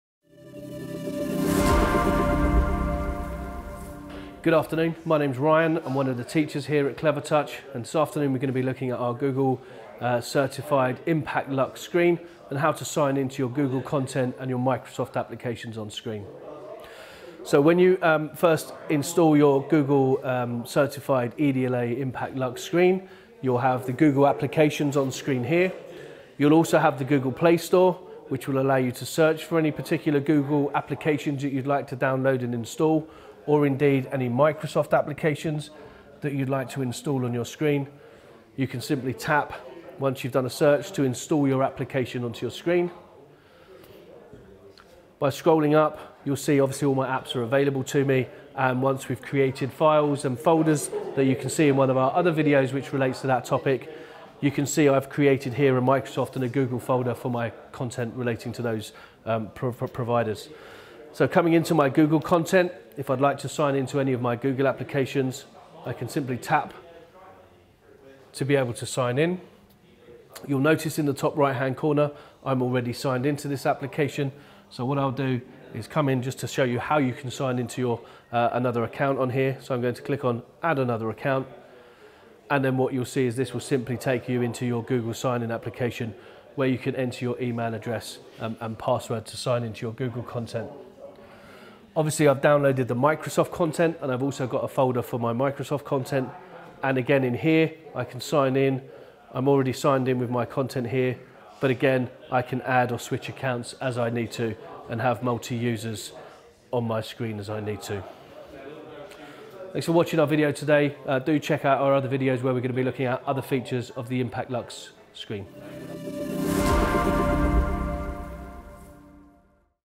IMPACT Lux Tutorial | Signing in to your Google or Microsoft accounts | Clevertouch
Designed to work with Microsoft or Google. Here's how to log in to your accounts and access your files on the IMPACT Lux.